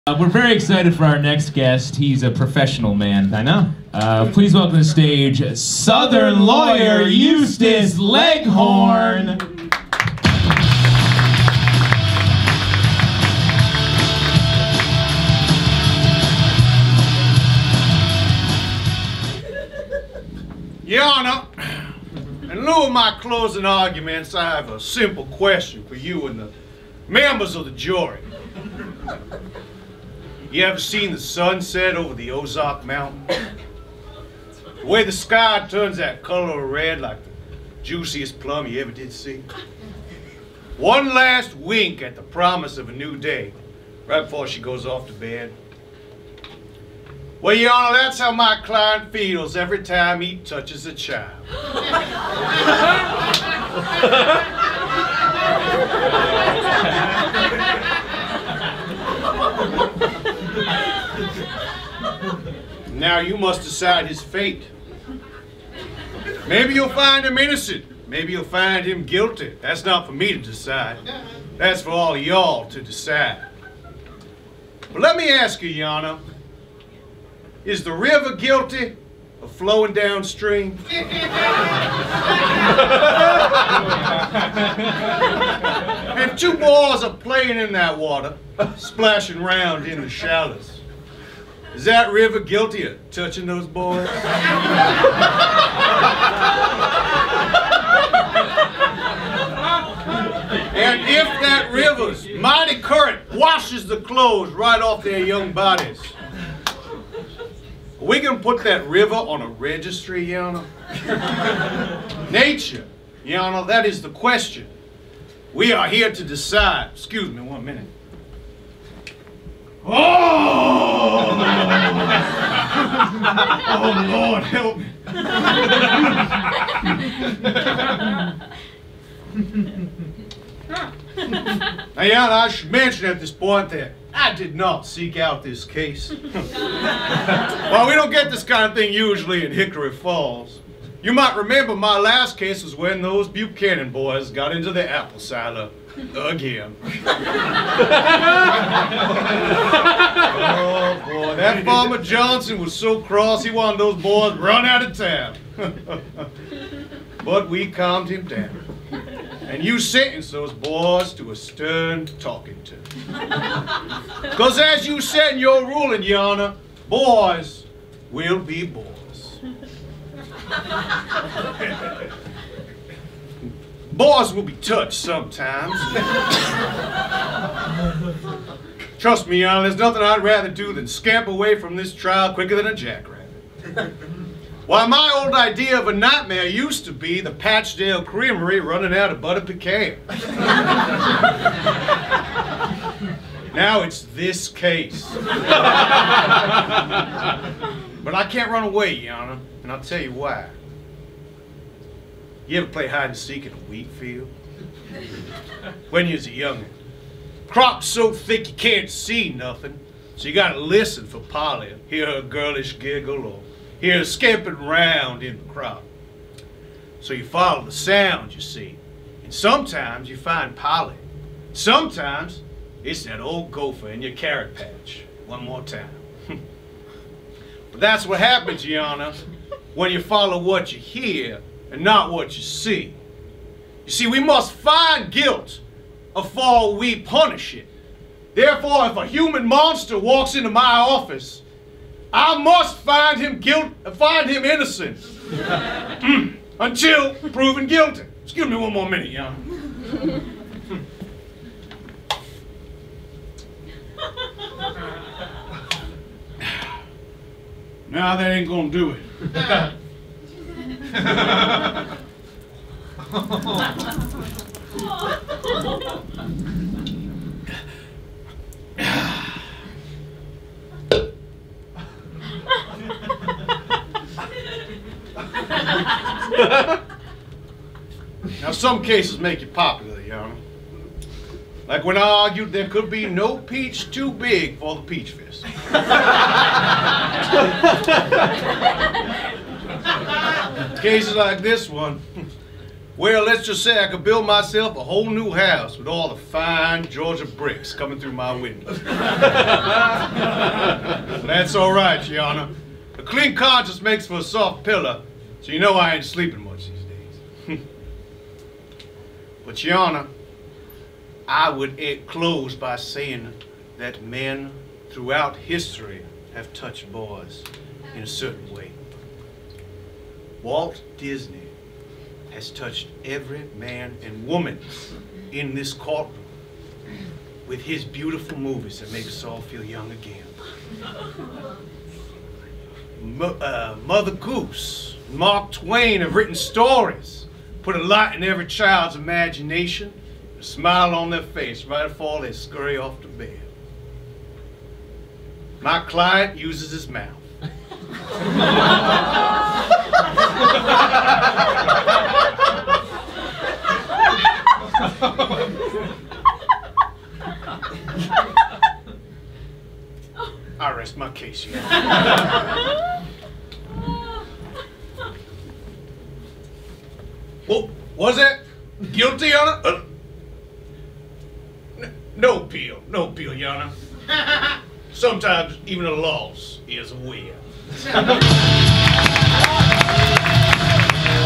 Southern Lawyer Eustace Leghorn
We don't see this kind of case much in Hickory Falls...

Written and performed by Jon Bander
Directed by Michael Hartney and Justin Tyler
Originally performed at Characters Welcome on 2/16/15 at Upright Citizens Brigade Theatre East
Shot and edited by Lou Gonzalez